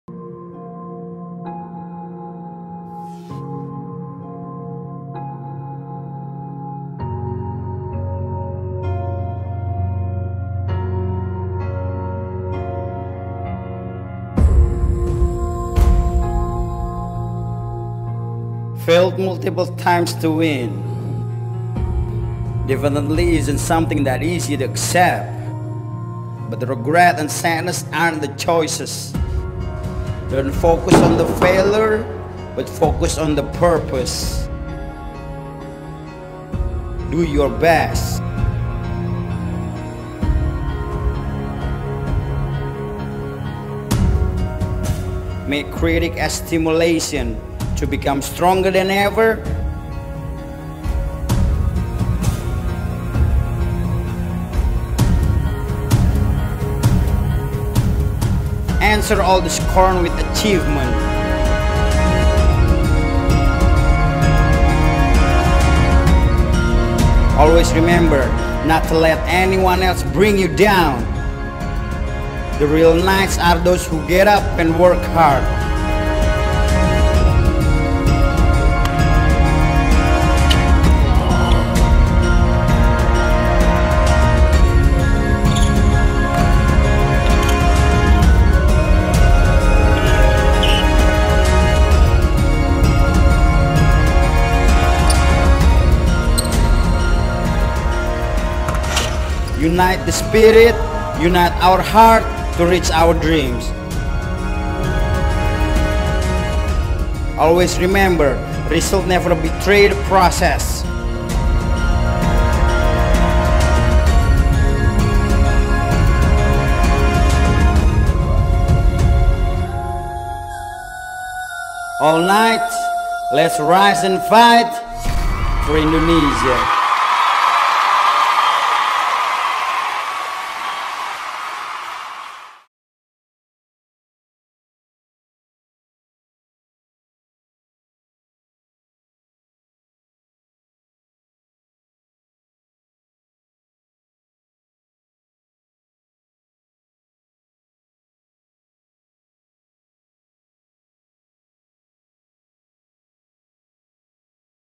IT'S TIME TO WIN! - CLS KNIGHTS
Setelah mengalami tiga kekalahan berturut turut CLS KNIGHTS yakin bahwa semua itu merupakan ujian dan pelajaran berarti. dan kami siap untuk berjuang lebih keras lagi menghadapi game - game selanjutnya.